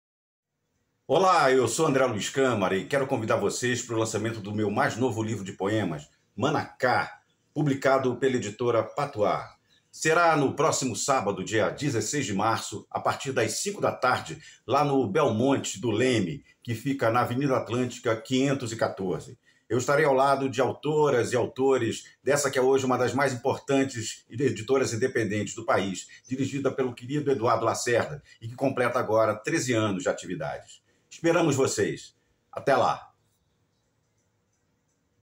Lançamento do meu novo livro, Manacá
O evento será no próximo sábado, 16/3, a partir das 17h, no Boteco Belmonte do Leme (Av. Atlântica, 514). Uma publicação da Patuá @editorap, editora que comemora 13 anos de atividades